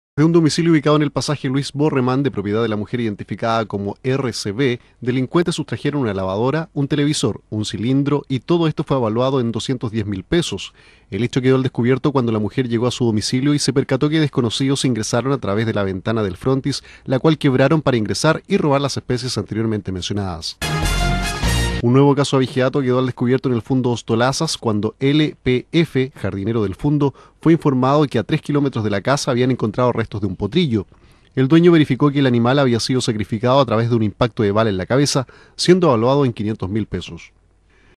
RESUMEN POLICIAL
ROBAN DESDE DOMICILIO.

Desde un domicilio ubicado en el pasaje Luis Borreman de propiedad de la mujer identificada como RCV, delincuentes sustrajeron una lavadora, un televisor y un cilindro, todo esto fue avaluado en $210.000.
El hecho quedó al descubierto cuando la mujer llegó a su domicilio y se percató que desconocidos ingresaron a través de la ventana del frontis la cual quebraron para ingresar y robar las especies anteriormente mencionadas.

NUEVO CASO DE ABIGEATO.

Un nuevo caso de abigeato quedó al descubierto en el fundo Ostolazas cuando LPF, jardinero del fundo, fue informado que a tres kilómetros de la casa habían encontrado restos de un potrillo.
El dueño verificó que el animal había sido sacrificado a través de un impacto de bala en la cabeza.
Siendo avaluado en $500.000.